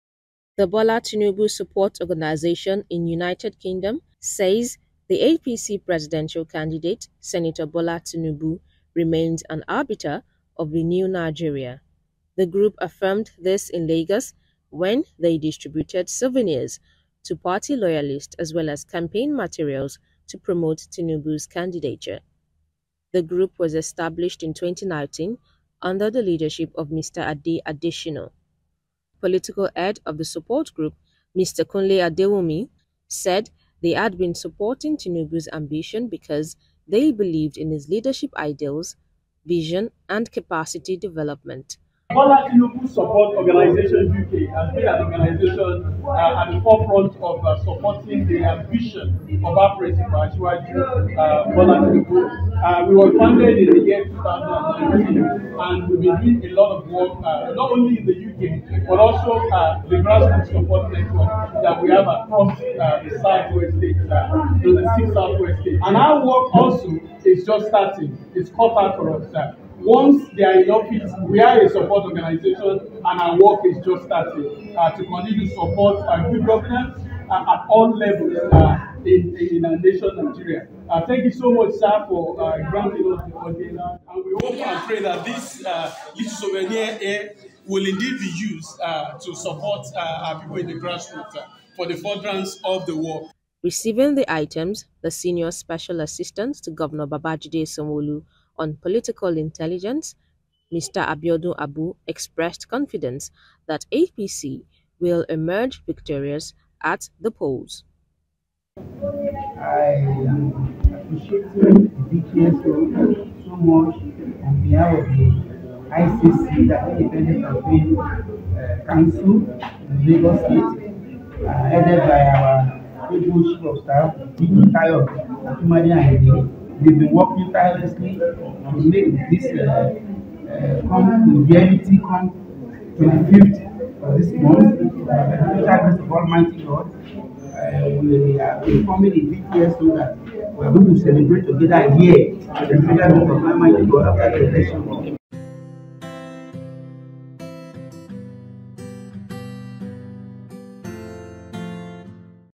Tinubu's UK supporters say APC candidate harbinger of new Nigeria
The Bola Tinubu Support Organisation in United Kingdom (BTSO UK) says the APC Presidential Candidate, Sen. Bola Tinubu remains an harbinger of a new Nigeria.

The group affirmed this in Lagos when they distributed souvenirs to party loyalists as well as campaign materials to promote Tinubu's candidature